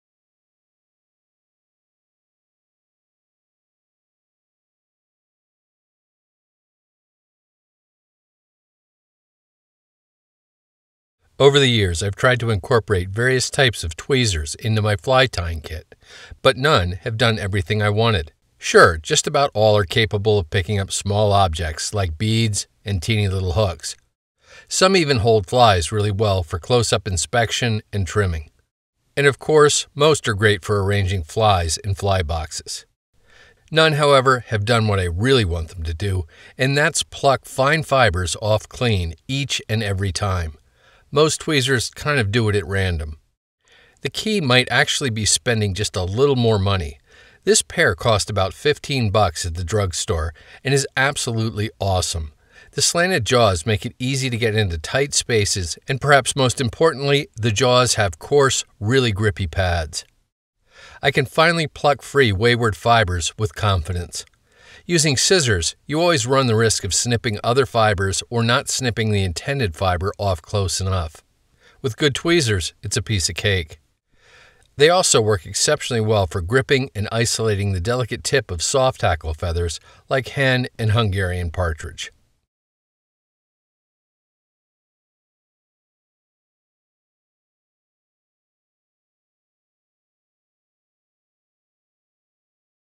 Tweezers
Video illustrates the uses of quality tweezers in fly tying.